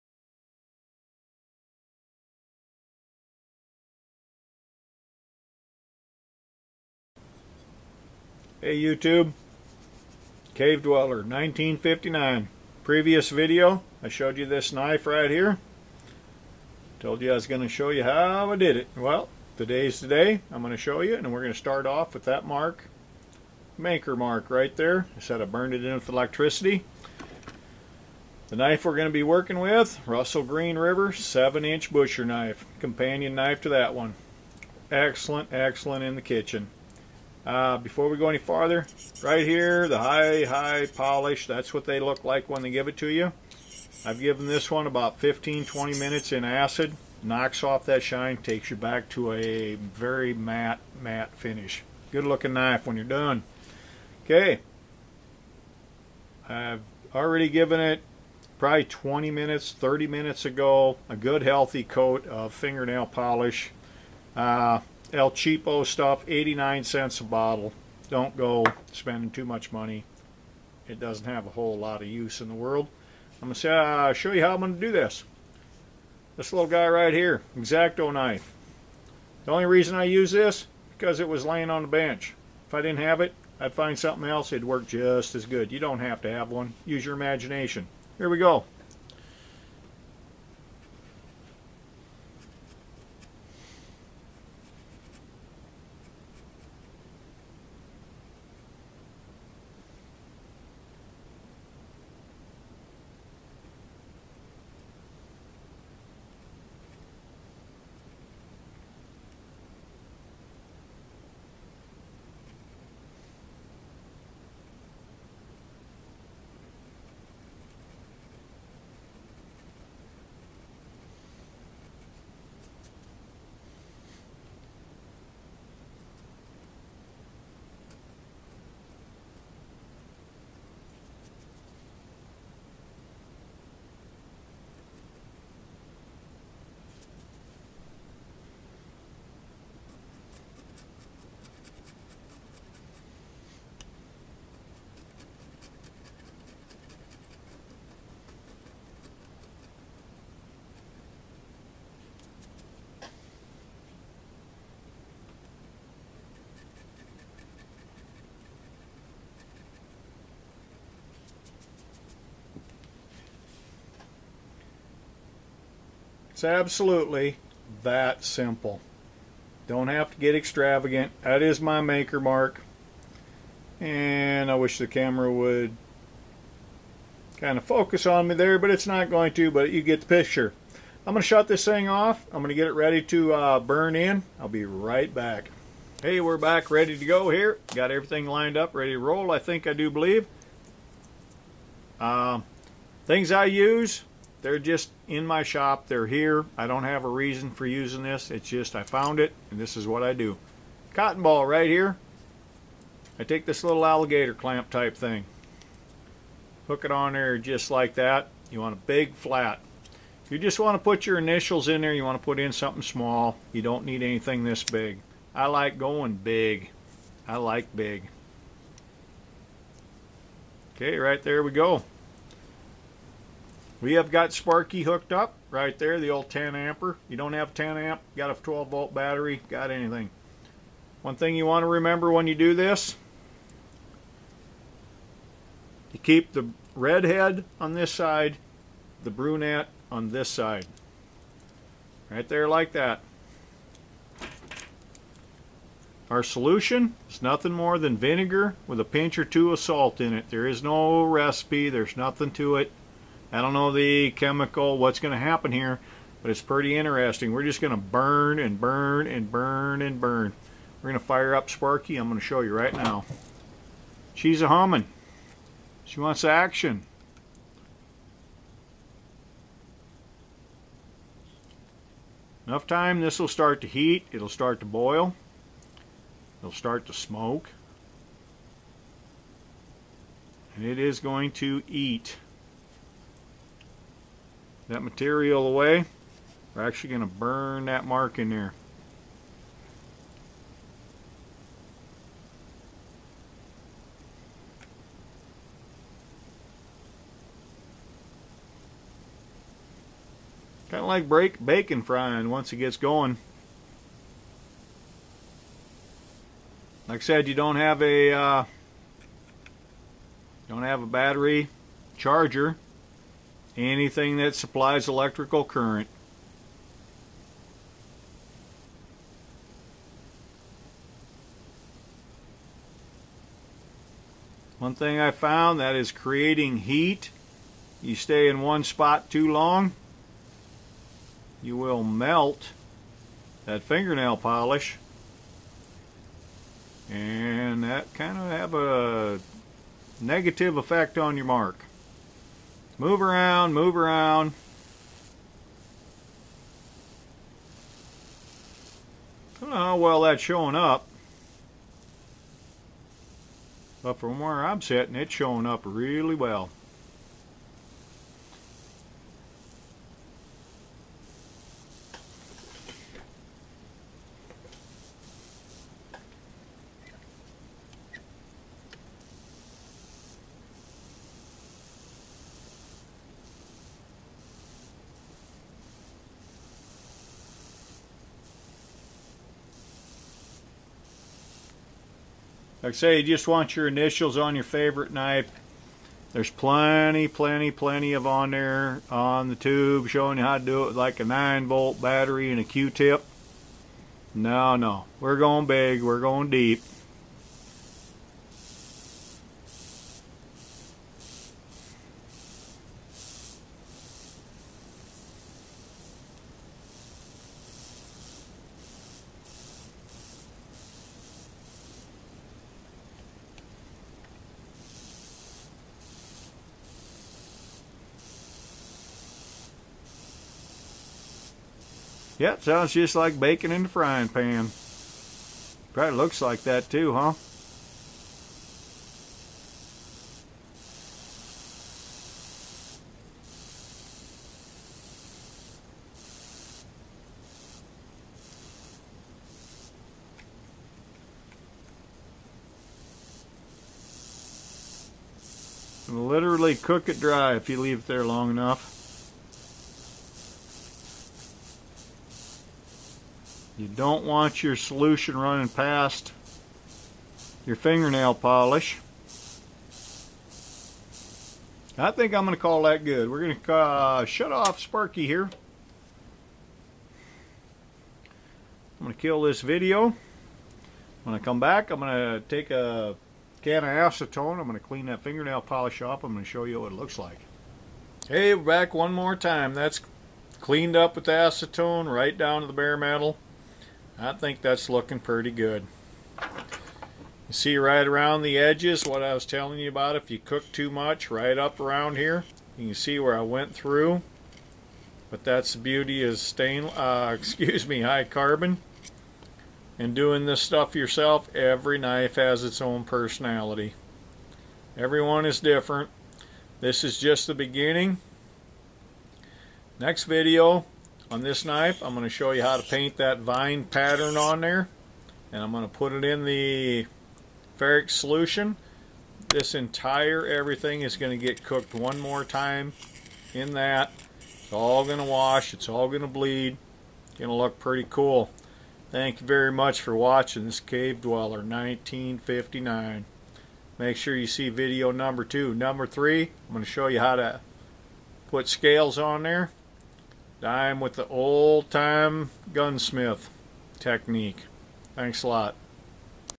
ACID ETCHING KNIVES. " HOW TO" MAKE YOUR MARK..
Names, dates, Happy Birthday, ANYTHING YOU CAN THINK OF.
No limit to what you can do to customize your blades